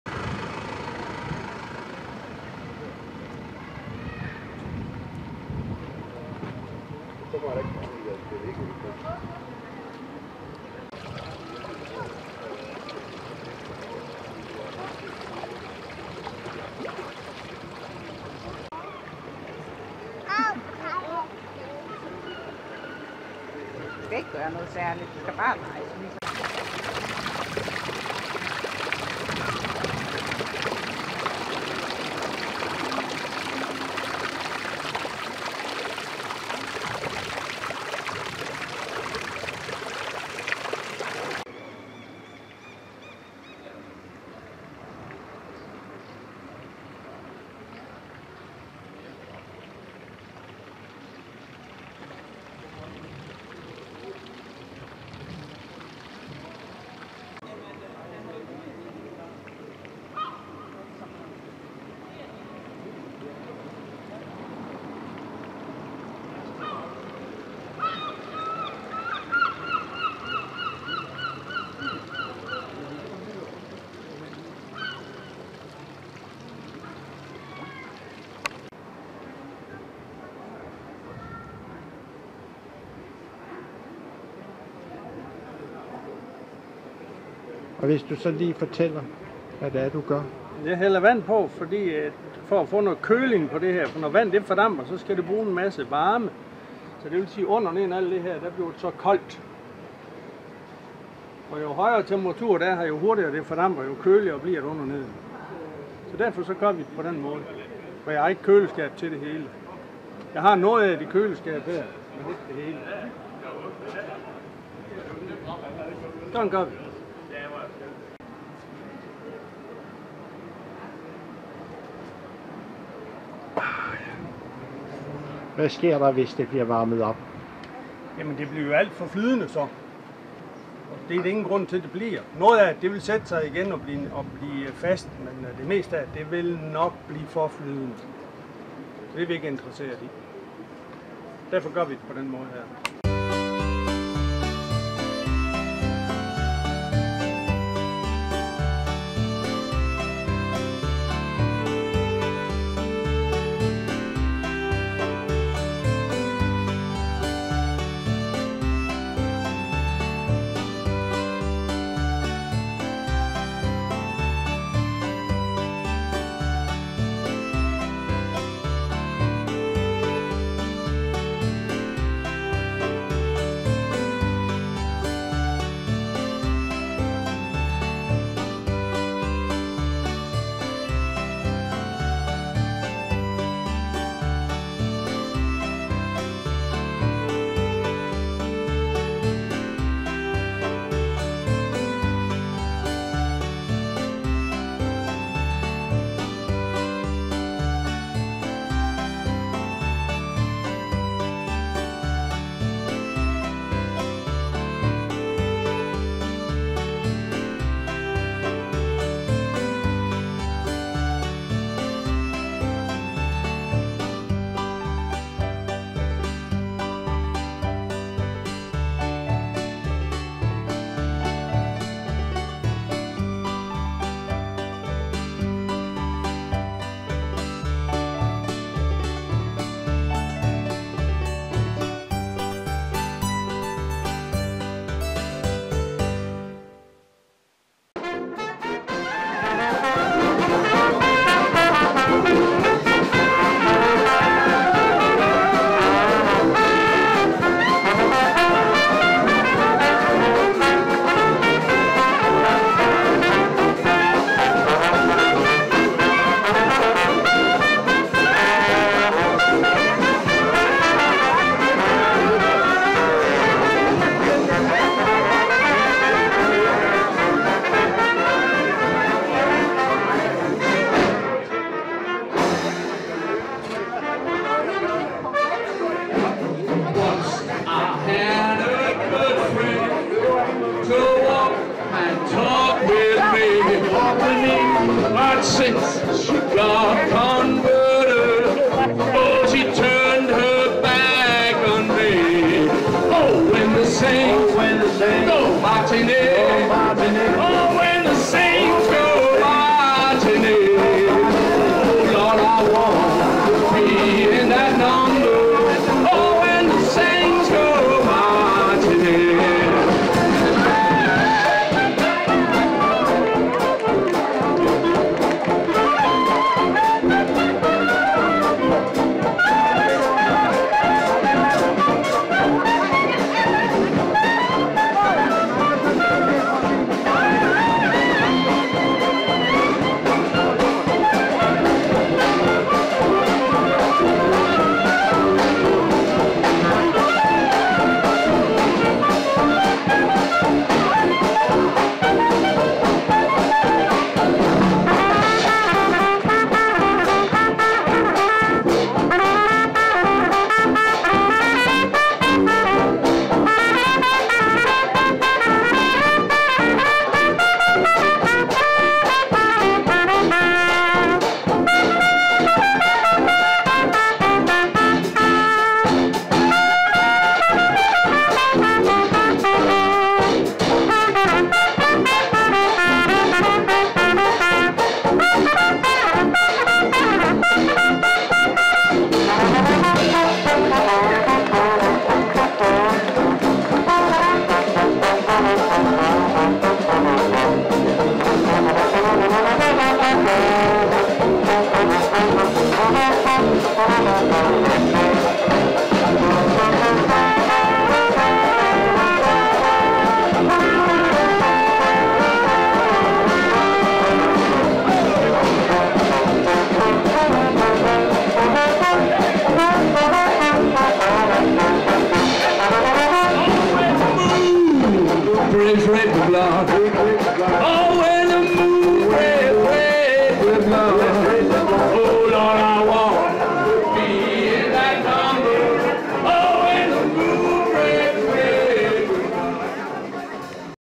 Fra Torvet til Springvand
SØNDERGADE HORSENS - Lørdag den 25. juni 2022 - Fra Torvet til Springvandet.
Mange horsensborgere og turister nyder at gå en tur på Søndergade, der er blevet renoveret for flere millioner kroner.
Der er mange skulpturer og levende musik kan opleves i Apotekergården.